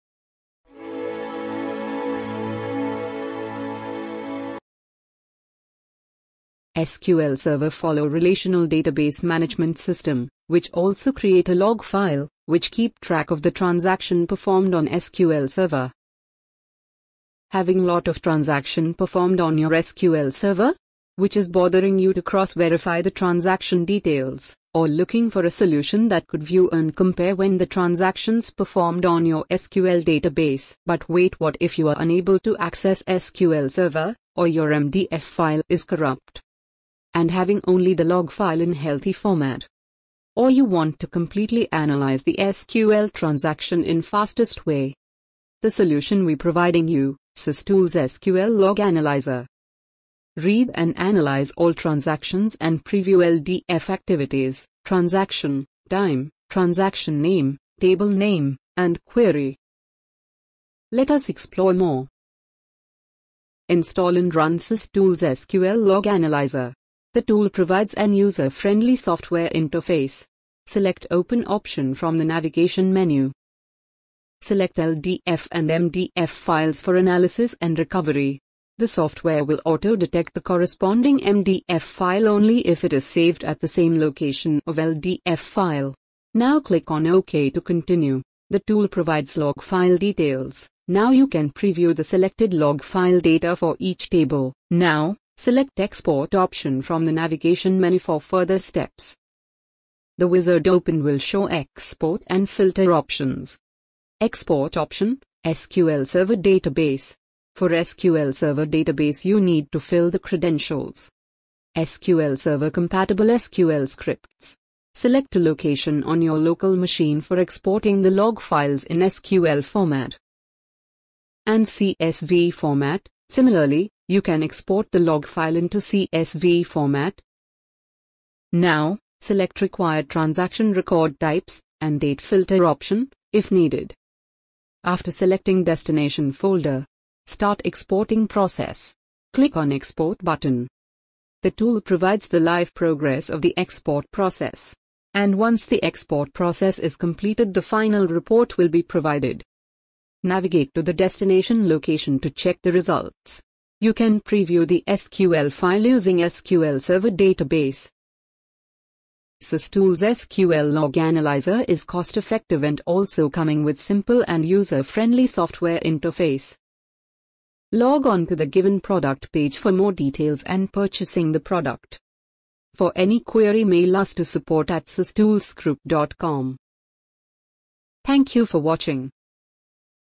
How to View SQL Server Transaction Log
Learn How to View SQL Server Transaction Log File using SysTools Log Analyzer Tool. More
The software is a standalone tool to view the SQL transaction log.  Now you can open, read & analyze SQL server transactions. And also the tool repair SQL transaction log file easily.  SQL Server Log file analyzer promotes the forensic analysis of Log files for the examination of SQL Server activities. The tool works on offline SQL database only and  MDF File is required to analyze the database.